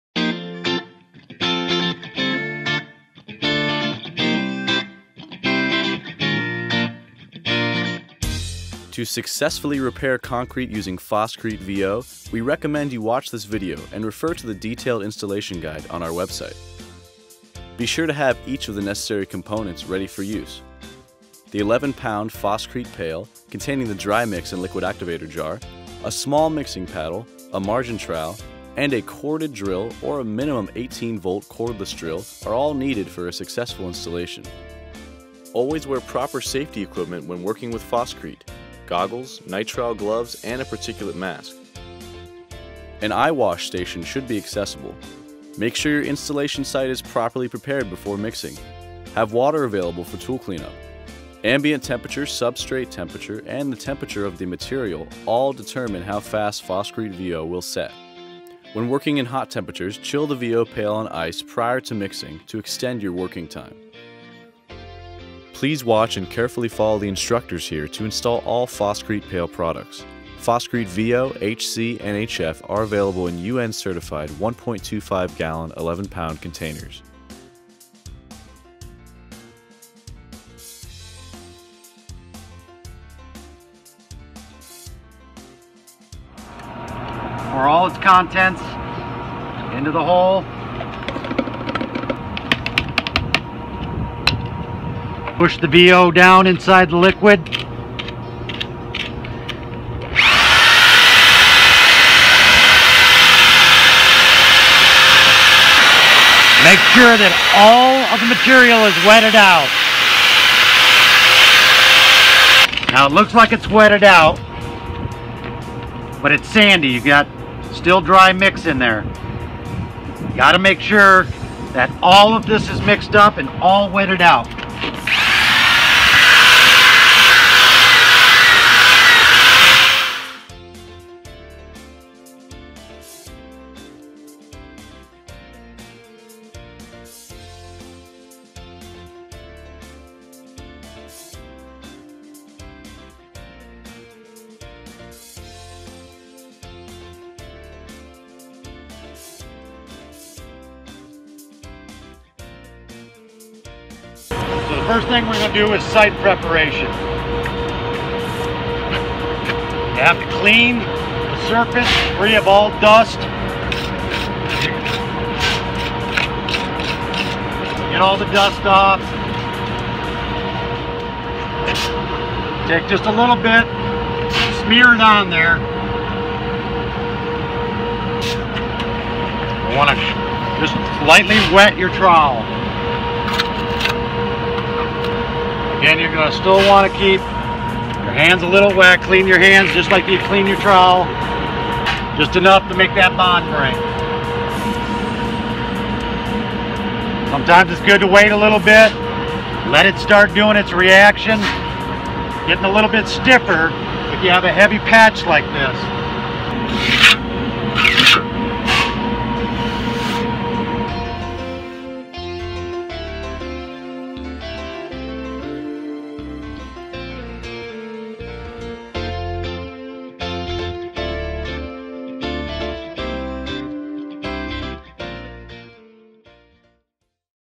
Phoscrete VO Installation
Watch and learn how to mix, place, and finish Phoscrete VO for Vertical and Overhead concrete repairs. Phoscrete HC is also available as a pail product for Horizontal and Castable repairs (including freezer floors)